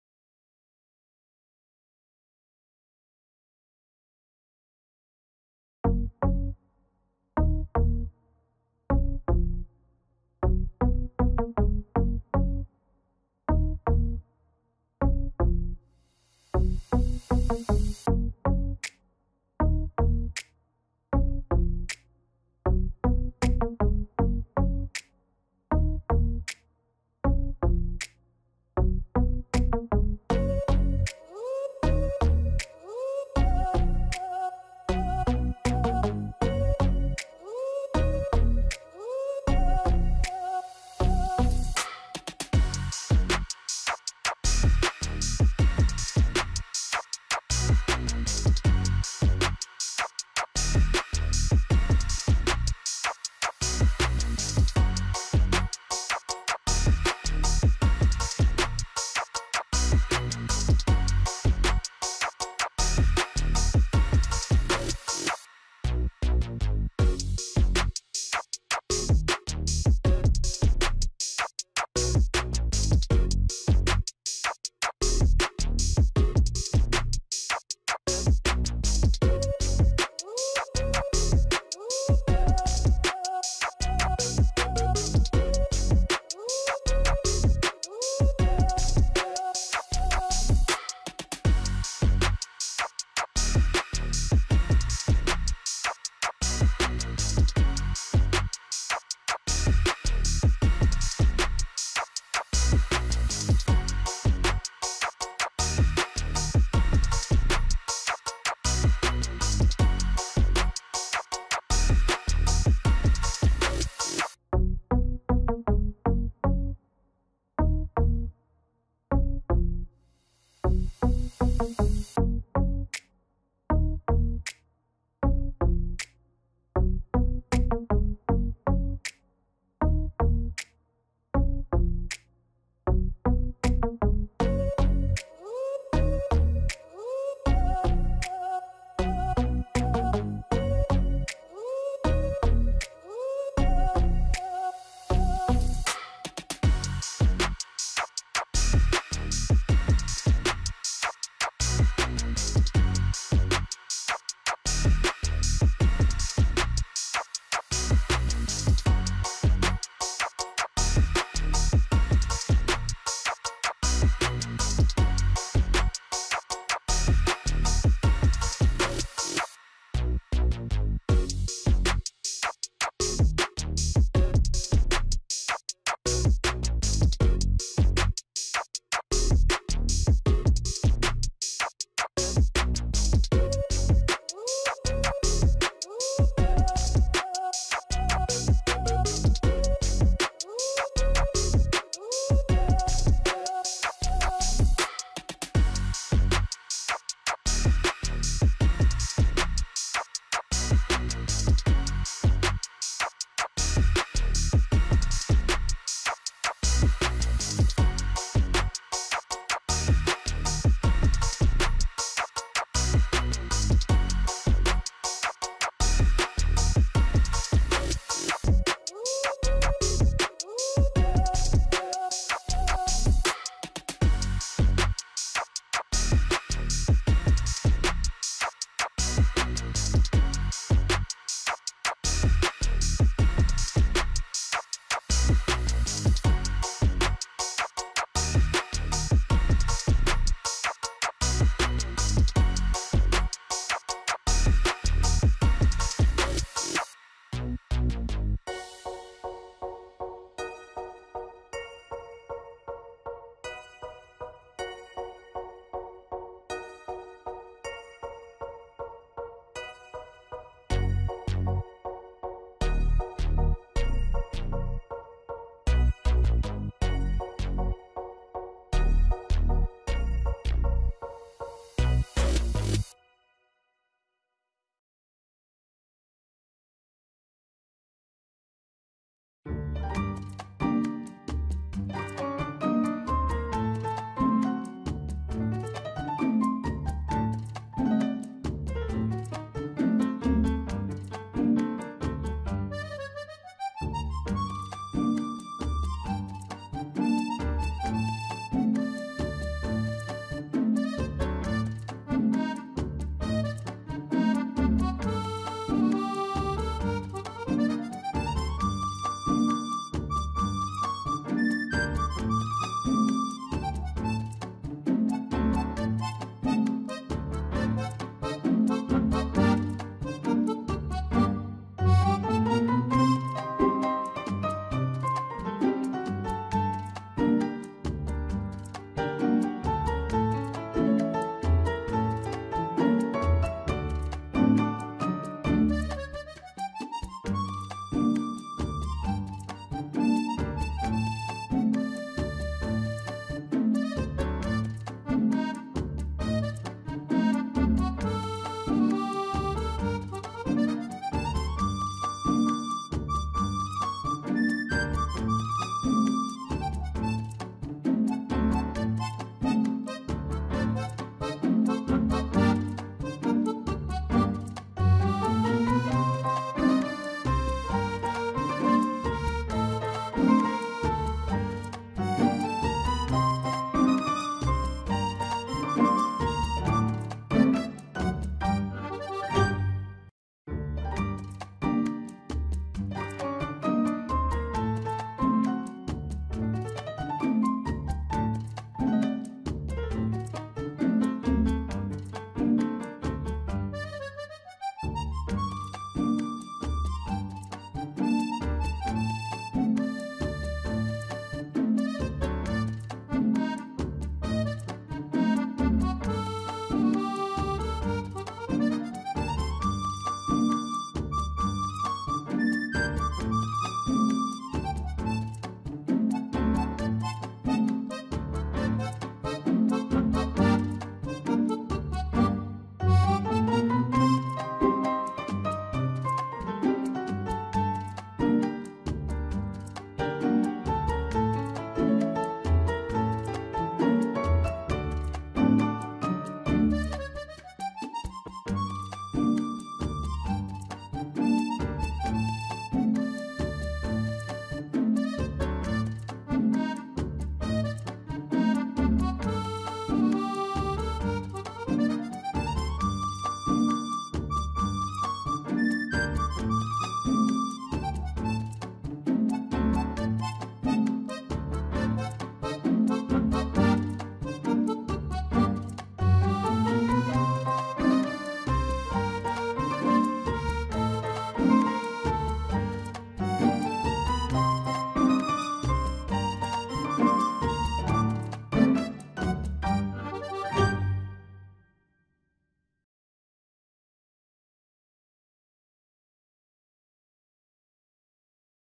Realme GT Neo 2 vs Poco X3 GT | SpeedTest and Camera comparison
Realme GT Neo 2 5G Dual 256GB 12GB RAM Factory Unlocked (GSM Only | No CDMA - not Compatible with Verizon/Sprint) International Version -Neo Black